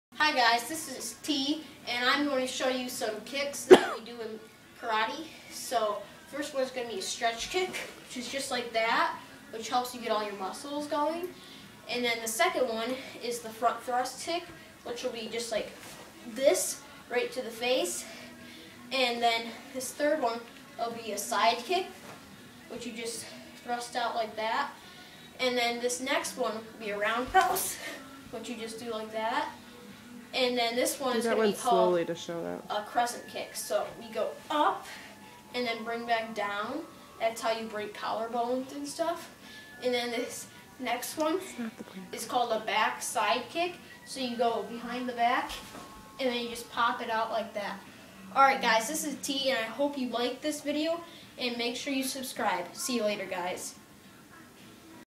Karate Kicks
These are some kicks that people who do karate use